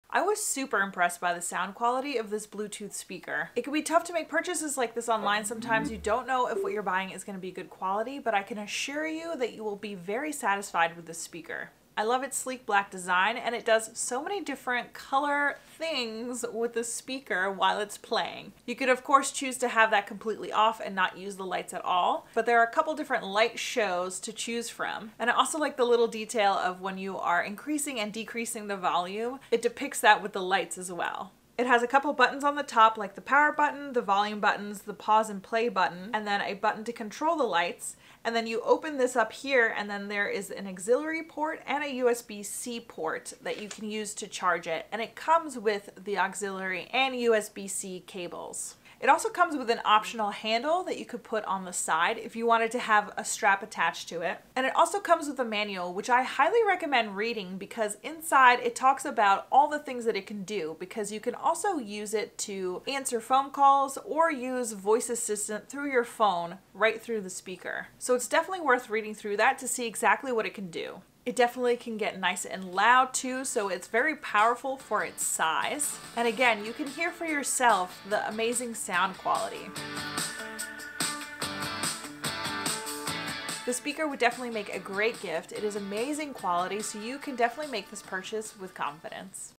🔊Elevate Your Music Experience with the Portable Bluetooth Speaker!🌊🎶
Get ready to immerse yourself in stunning sound quality and vibrant visuals with the IPX7 Waterproof Wireless Speaker. This portable speaker delivers 25W of super bass, ensuring a rich and immersive audio experience that will leave you mesmerized. Designed for adventure, this speaker boasts an IPX7 waterproof rating, allowing you to enjoy your favorite tunes even by the poolside or at the beach. No need to worry about splashes or rain showers—the speaker can handle it all.

But it's not just about the sound—the colorful flashing lights add an extra element of fun to your music experience. Watch as the lights dance and groove to the rhythm of your favorite tracks, creating a captivating visual spectacle. With a whopping 24 hours of playtime, you can enjoy non-stop music throughout the day and night. The 100ft Bluetooth range provides seamless connectivity, allowing you to move around freely without losing your connection to the speaker. Want an even more immersive experience? Take advantage of the TWS pairing feature and pair two speakers together for true stereo sound. Create a surround sound effect that will bring your parties and gatherings to life. Whether you're at home, on a road trip, or chilling at the beach, this portable Bluetooth speaker is your perfect companion. Its compact and travel-friendly design allows you to take it anywhere you go, ensuring that the music never stops.

🛍️Shop by Clicking the Link Below to Find it on Amazon now:
*The one featured is not currently available, but we've linked a similar (Amazon Favorite) one for you!